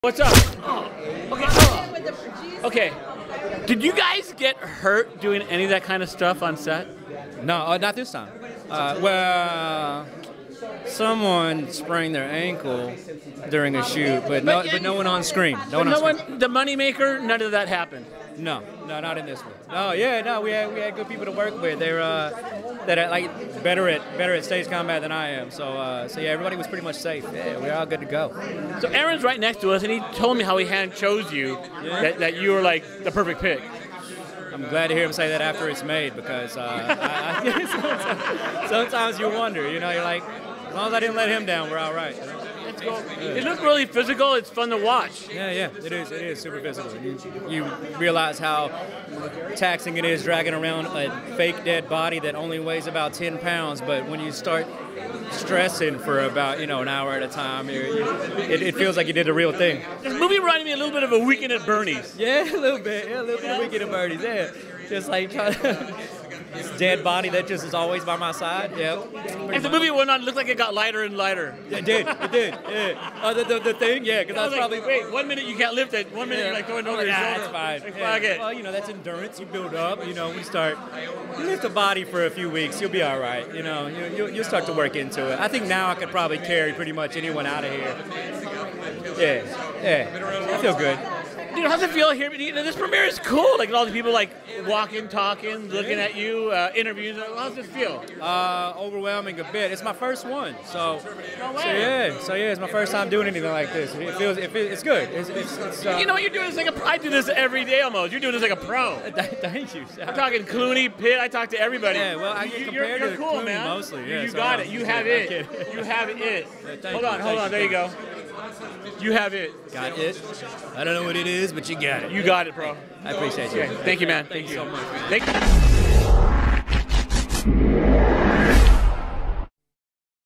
Actor Cael Adcock details his latest creation "Bury Him"
"Bury Him"
After his controversial acquittal for murder, Michael Kenny must lay low. This becomes difficult after accidentally killing a man with his car.